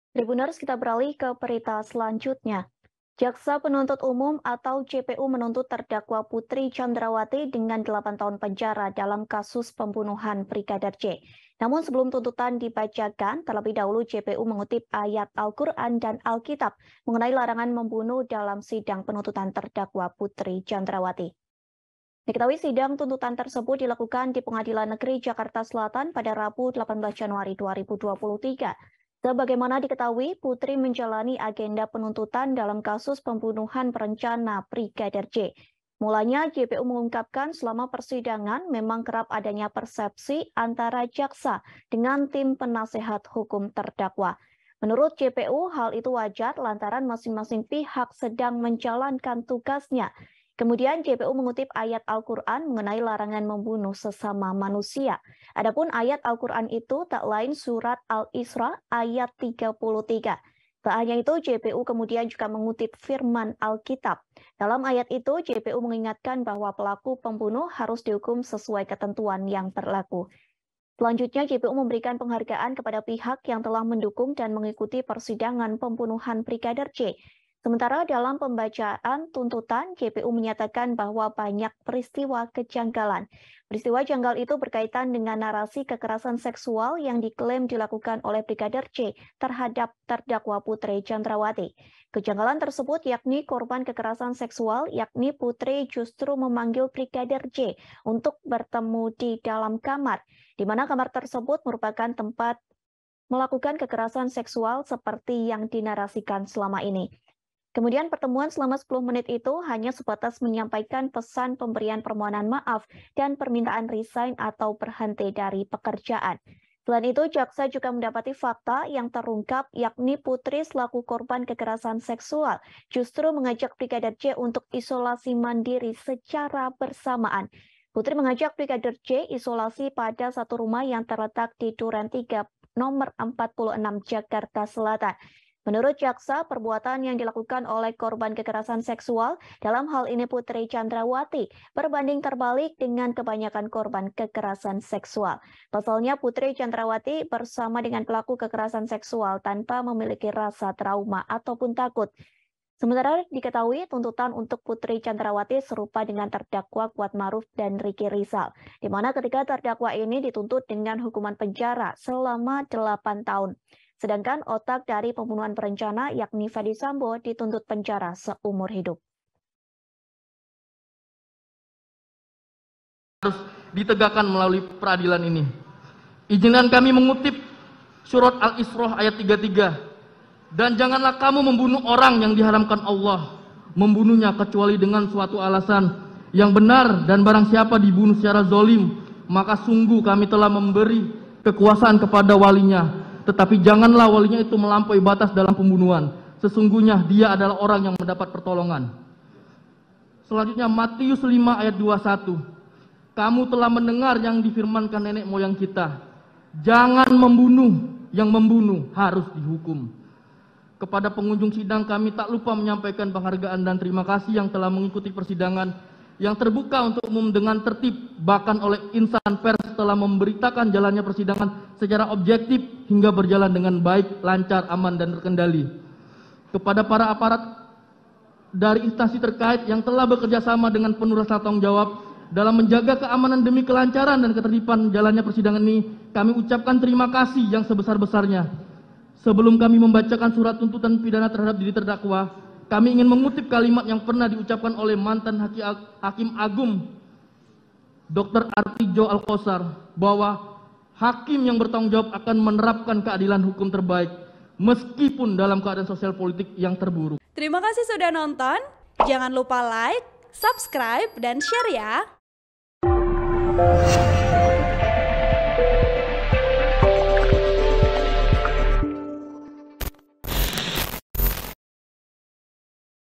Jaksa 'Bawa-bawa' Ayat Alquran & Alkitab Sebelum Bacakan Tuntutan Hukuman PC 8 Tahun Penjara
TRIBUN-VIDEO.COM - Jaksa Penuntun Umum (JPU) menuntut terdakwa Putri Candrawathi dengan delapan tahun penjara dalam kasus pembunuhan Brigadir J.

Namun sebelum tuntutan dibacakan, terlebih dahulu JPU mengutip ayat Alquran dan Alkitab mengenai larangan membunuh dalam sidang penuntutan terdakwa Putri Candrawathi.

Diketahui sidang tuntutan tersebut dilakukan di Pengadilan Negeri Jakarta Selatan pada Rabu (18/1/2023).

Sebagaimana diketahui, Putri menjalani agenda penuntutan dalam kasus pembunuhan berencana terhadap Brigadir Yosua Hutabarat alias Brigadir J.

Mulanya, JPU mengungkapkan selama persidangan memang kerap adanya persepsi antara Jaksa dengan tim penasihat hukum terdakwa.

Menurut JPU, hal itu wajar lantaran masing-masing pihak tengah menjalankan tugasnya.

Kemudian, JPU mengutip ayat Alquran mengenai larangan membunuh sesama manusia.

Adapun ayat Alquran itu tak lain surat Al-isra ayat 33.

Tak hanya itu, JPU kemudian juga mengutip firman Alkitab.

Dalam ayat itu, JPU mengingatkan bahwa pelaku pembunuh harus dihukum sesuai ketentuan yang berlaku.

Selanjutnya, JPU memberikan penghargaan kepada pihak yang telah mendukung dan mengikuti persidangan pembunuhan Brigadir J.

Sementara dalam pembacaan tuntutan, JPU menyatakan bahwa banyak peristiwa kejanggalan.

Peristiwa janggal itu berkaitan dengan narasi kekerasan seksual yang diklaim dilakukan oleh Brigadir J terhadap terdakwa Putri Candrawathi.

Kejanggalan tersebut yakni korban kekerasan seksual yakni Putri Candrawathi justru memanggil Brigadir J untuk bertemu di dalam kamar.

Di mana kamar tersebut merupakan tempat kekerasan seksual seperti yang dinarasikan selama ini.

Kemudian pertemuan selama 10 menit itu hanya sebatas menyampaikan pesan pemberian permohonan maaf dan permintaan resign atau berhenti dari pekerjaan.

Selain itu, jaksa juga mendapati fakta yang terungkap yakni Putri selaku korban kekerasan seksual justru mengajak Brigadir J untuk isolasi mandiri secara bersamaan.

Putri mengajak Brigadir J isolasi pada satu rumah yang terletak di Duren Tiga Nomor 46, Jakarta Selatan.

Menurut jaksa, perbuatan yang dilakukan oleh korban kekerasan seksual dalam hal ini Putri Candrawathi berbanding terbalik dengan kebanyakan korban kekerasan seksual.

Pasalnya Putri Candrawathi bersama dengan pelaku kekerasan seksual tanpa memiliki rasa trauma dan takut.

Sementara diketahui tuntutan untuk Putri Candrawathi serupa dengan terdakwa Kuat Maruf dan Ricky Rizal.

Di mana ketiga terdakwa ini dituntut dengan hukuman penjara selama 8 tahun.

Sedangkan otak dari pembunuhan berencana yakni Ferdy Sambo dituntut penjara seumur hidup.

(Tribun-Video.com/Tribunnews.com)

Artikel ini telah tayang di Tribunnews.com dengan judul Jaksa Kutip Ayat Alquran dan Alkitab soal Larangan Membunuh di Depan Putri Candrawathi,